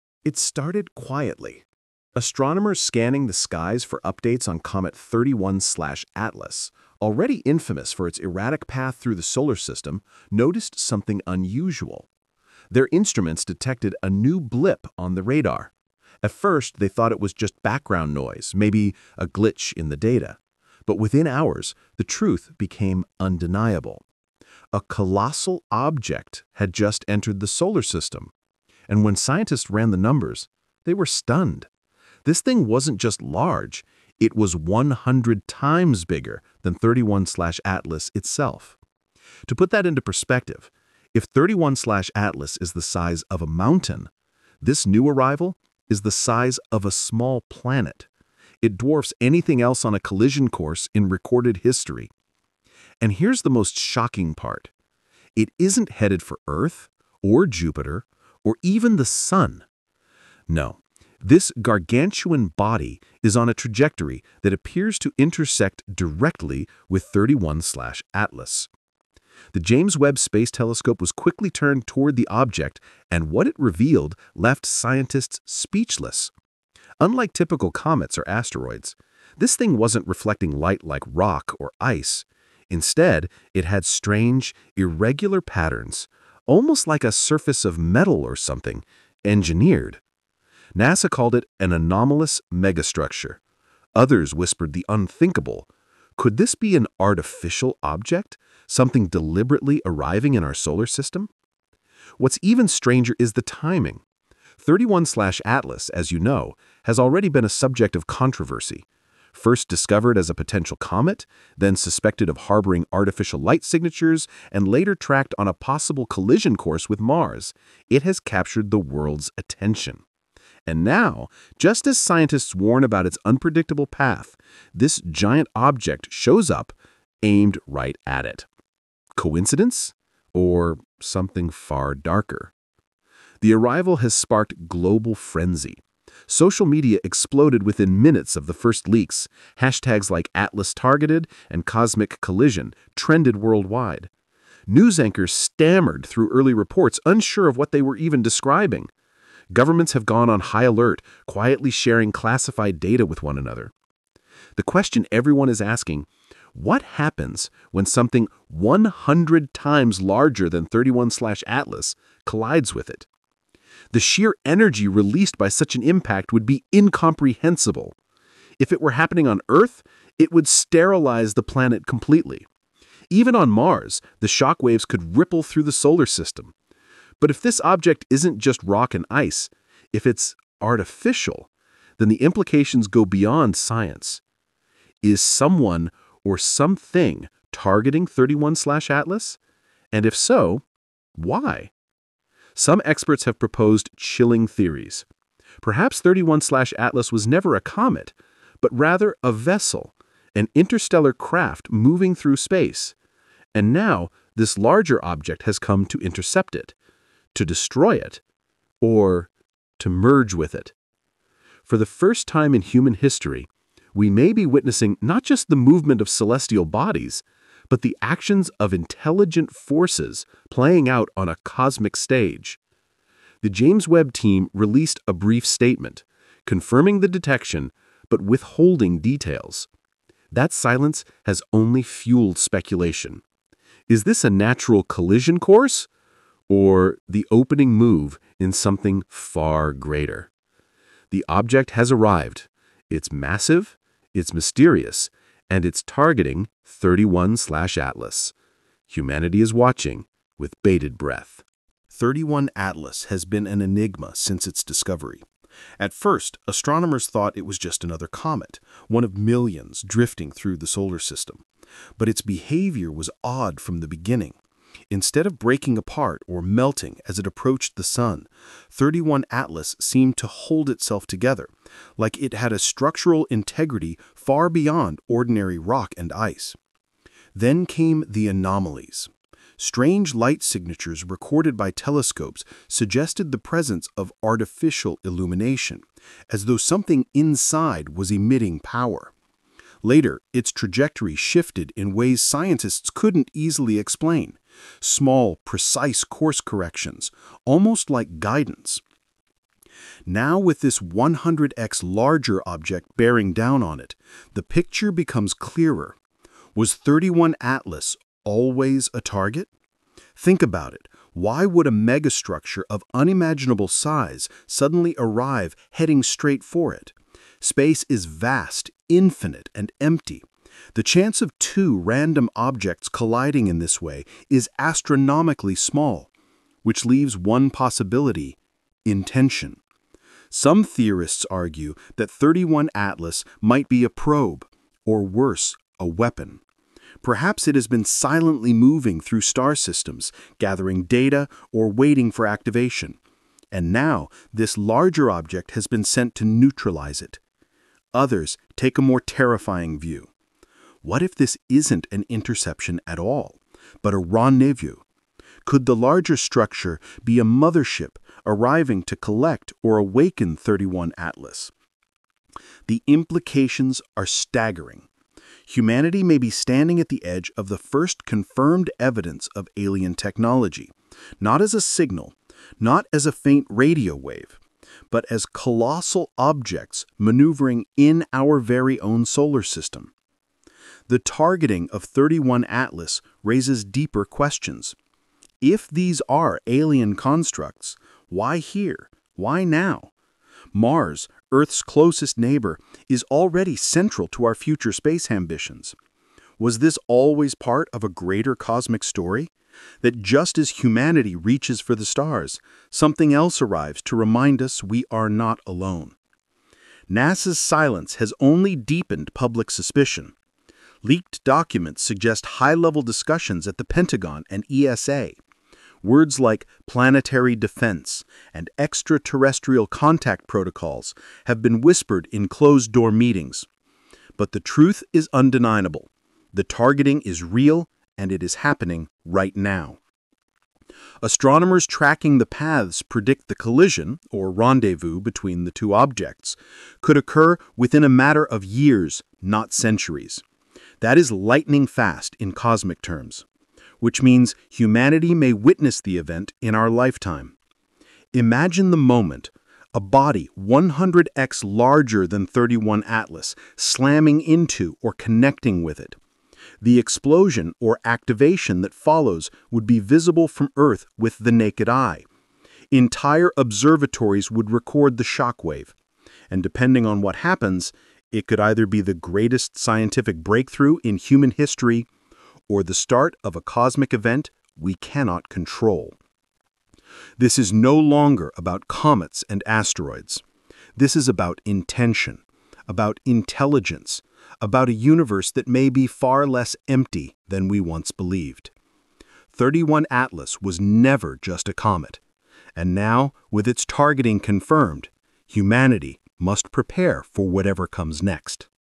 1 Minute Ago: 100x Bigger Object Just Arrived – Targeting 31/ATLAS!
Just 1 minute ago, astronomers confirmed the arrival of an object 100x larger than 31/ATLAS—and it’s heading directly toward it!

This isn’t just another comet or asteroid. The strange signals, artificial-looking reflections, and deliberate trajectory suggest something far more mysterious. Could this be alien technology? A cosmic weapon? Or the greatest scientific revelation in human history?

🌍 Humanity is watching as the largest object ever recorded in motion may collide with 31/ATLAS. What happens when they meet could change the course of history forever.